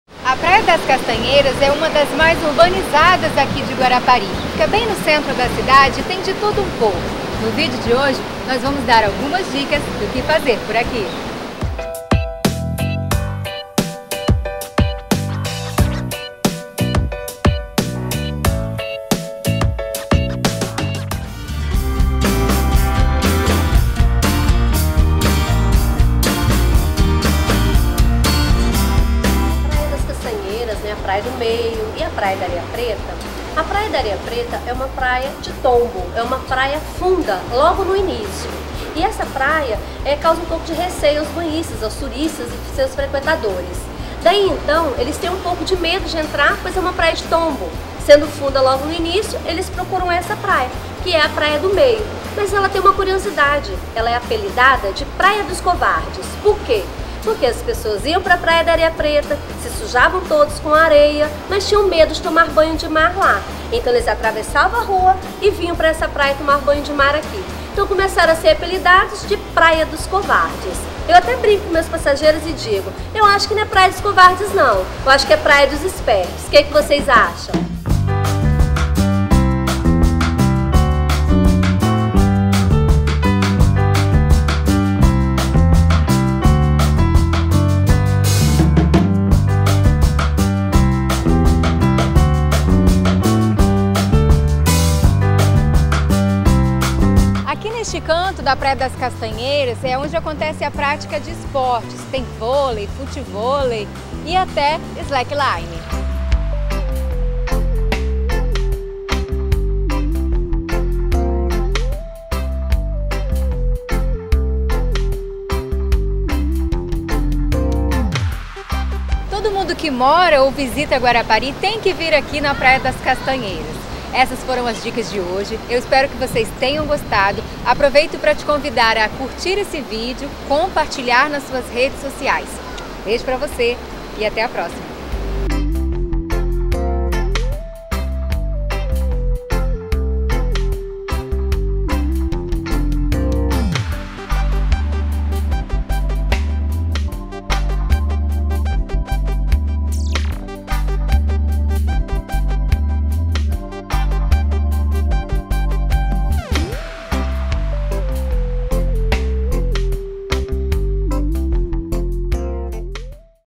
Praia das Castanheiras Guarapari
A Praia das Castanheiras está entre as mais urbanizadas aqui de Guarapari.
Tem de tudo um pouco. Está entre as preferidas para quem vem com
crianças.

As piscinas naturais que se formam entre as pedras e a areia são ótimas para quem leva os pequeninos na praia;

As árvores frondosas são um convite para se abrigar debaixo e se proteger do sol.